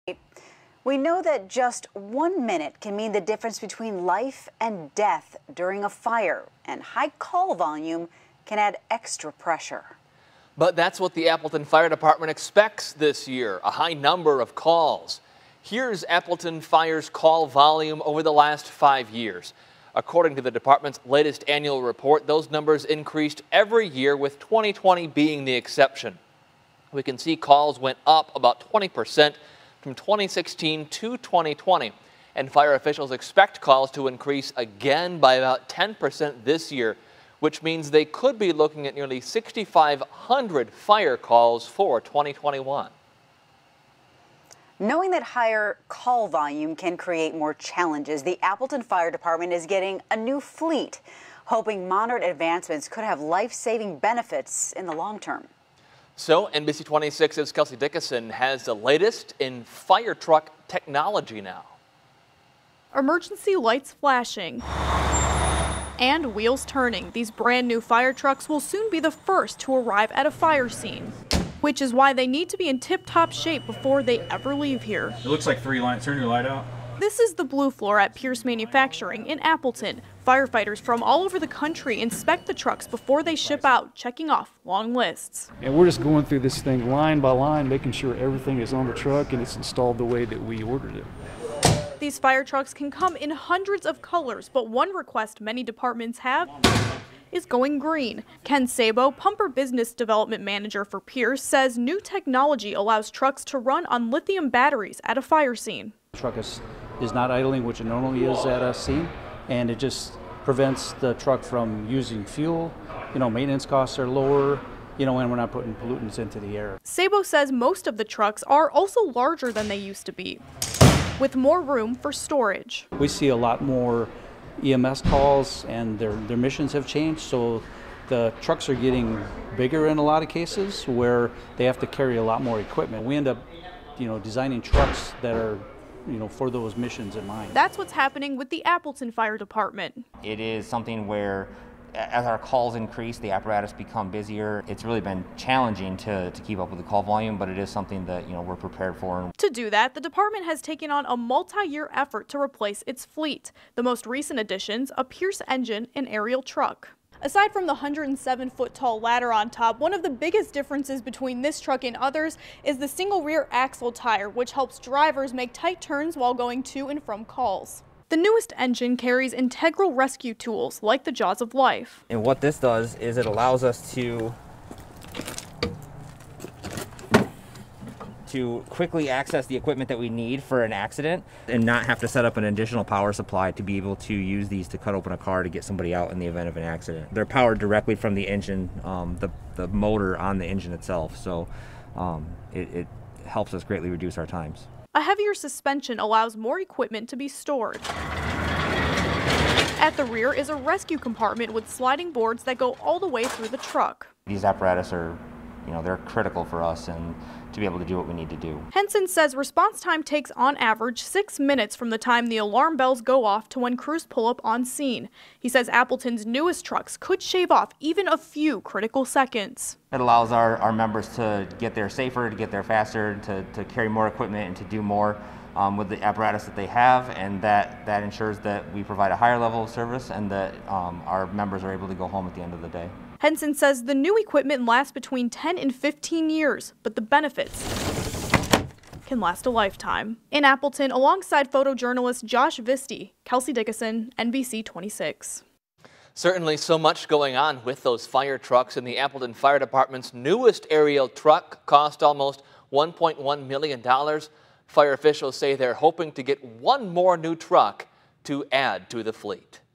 The latest in fire truck technology
How modern advancements in fire truck technology are benefiting the Appleton Fire Department.